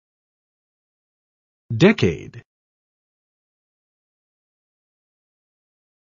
How to Pronounce Decade
This video shows you how to say Decade.